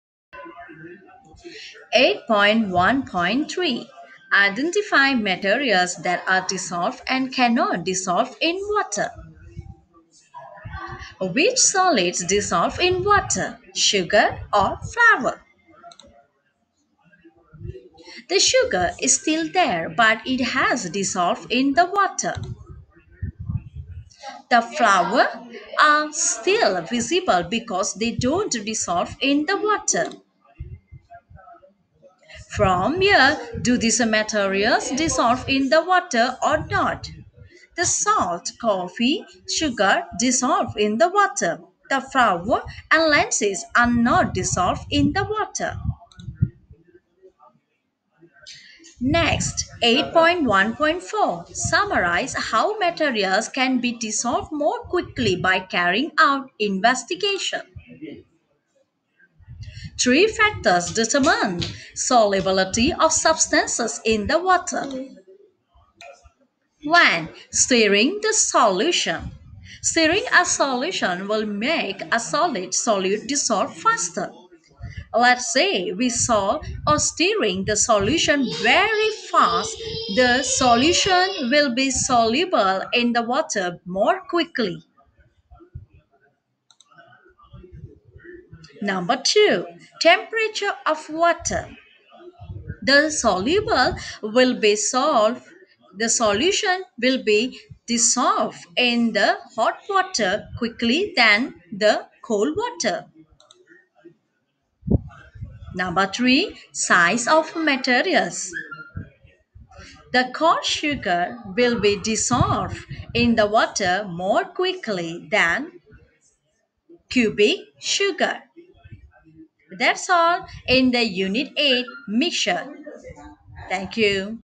UNIT 8 : MIXTURE (PART2) - DLP SCIENCE YEAR 2
Learn more about materials can dissolve and cannot dissolve in water. How do you can increase the rate of dissolving?